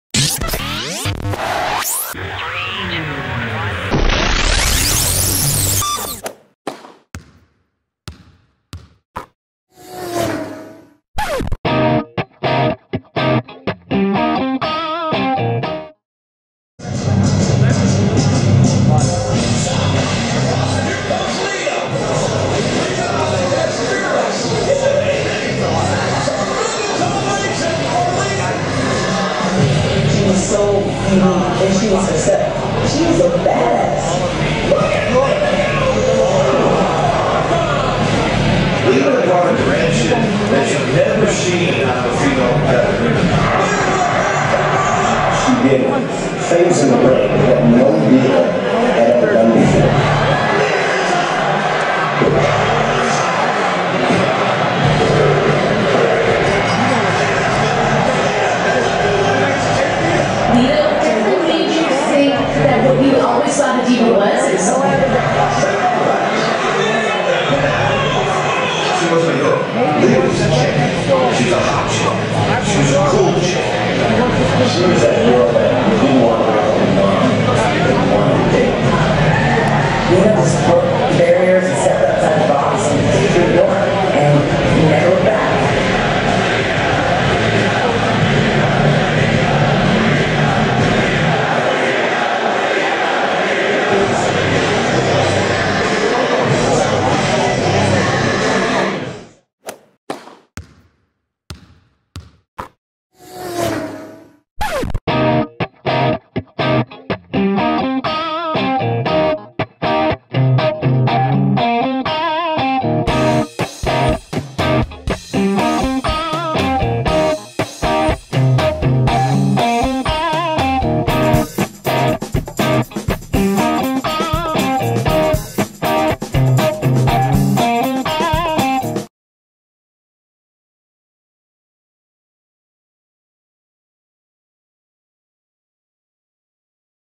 Lita professional wrestler and WWE Diva, to be inducted WWE Hall of Fame at WrestleMania 30 in Ne
Amy Christine Dumas best known by her stage name Lita, is a retired professional wrestler and WWE Diva active from 1999 to 2006.

After briefly working on the independent circuit and in Extreme Championship Wrestling, Dumas signed with World Wrestling Federation in 1999. Originally, she was paired with Essa Rios, but she achieved her greatest push alongside Matt and Jeff Hardy as Team Xtreme. Throughout her career, she won the WWE Women's Championship four times. During her tenure with the company she became arguably the most popular Diva in the company's history.